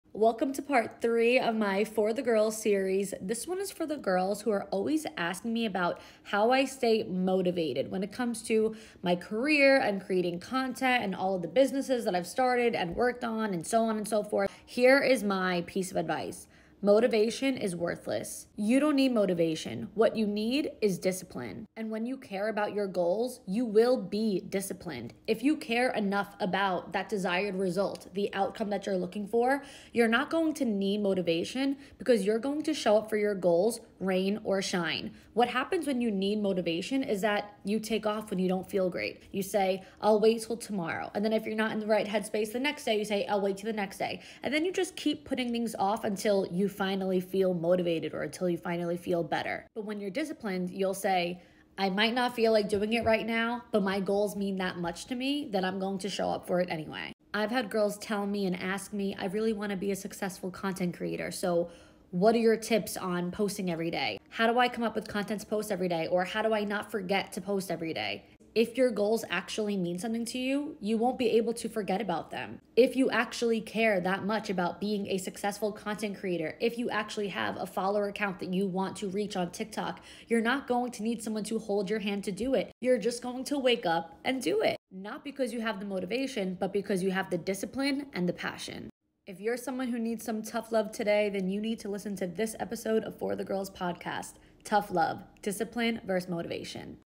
For The Girls - Tough Love: Discipline vs. Motivation
pI hope you&apos;re all ready for today&apos;s episode, girls. Big Vic Energy is coming out to play! This is the tough love you didn&apos;t want to hear but needed to hear. Have a financial goal? Stop looking for motivation. Have a fitness goal? Stop looking for motivation. Whatever goals you got, it&apos;s time to throw that motivational BS out the window and say hello to your new BFF, discipline. This quick gut check is going to get you ready to take on every goal you&apos;ve got!br/br/Be sure to follow For the Girls on IG at @forthegirls.podcast and my personal IG at @victoria.alario! Until next time, girls./p